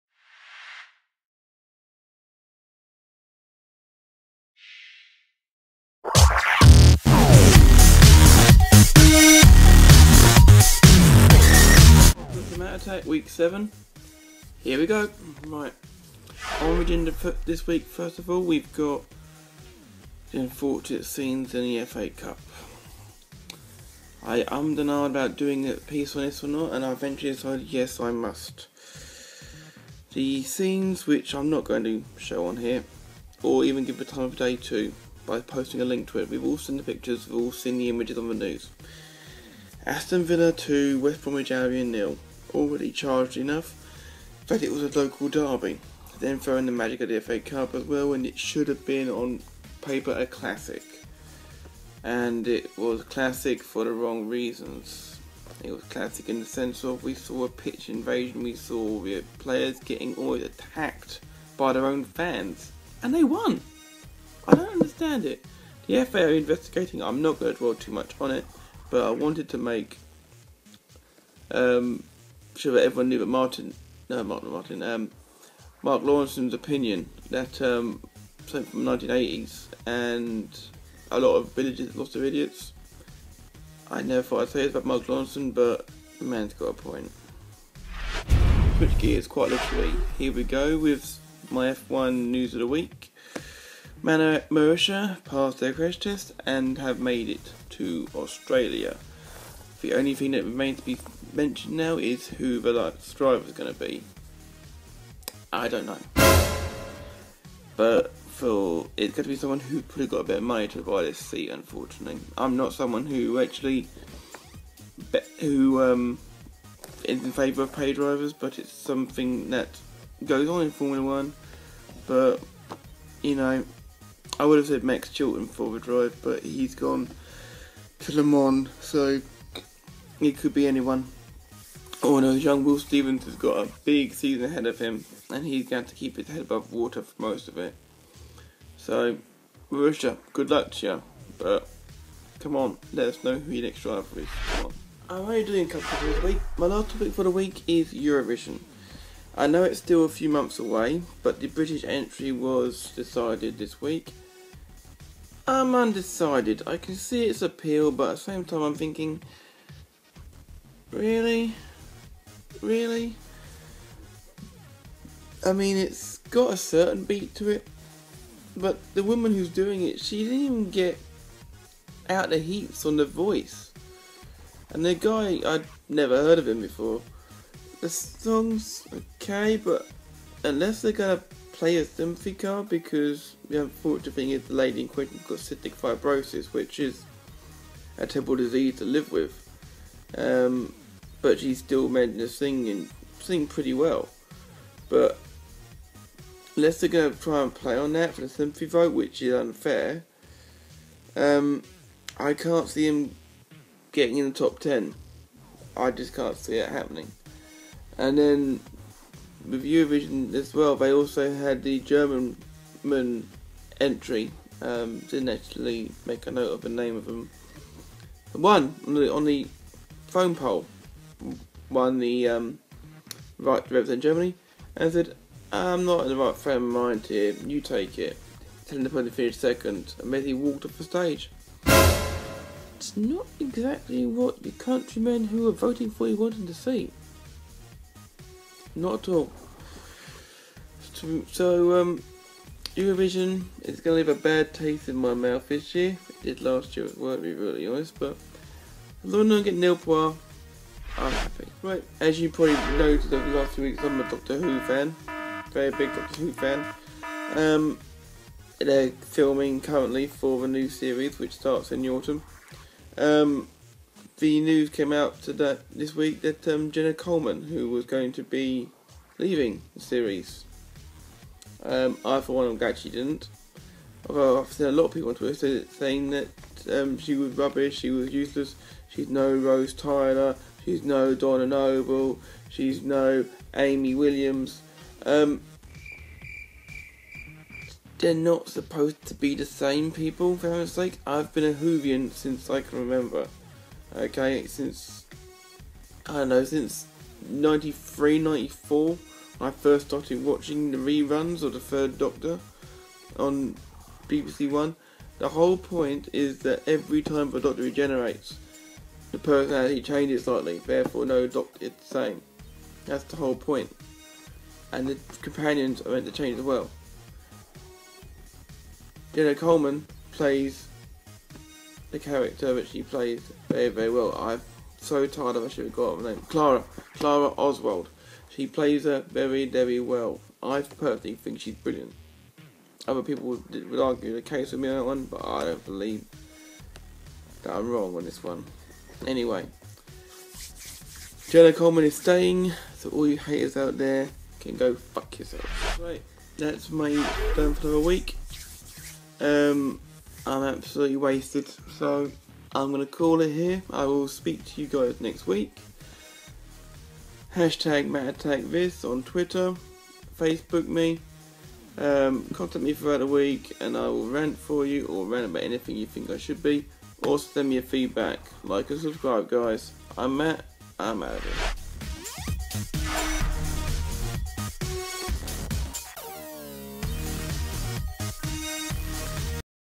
The Matt Attack - Episode 7 - 8th March 2015
This week,

I 'Matt Attack' the so-called Villa fans, Marussia not naming a second driver already, Eurovision two months early, and calling out the haters in defence of Jenna Coleman

This weeks episode is in loving memory of my Nana, who would've been 84 this week just gone. I miss you everyday and love you Nana

Twitter.com/TheMattAttackUK
Facebook.com/TheMattAttackOfficial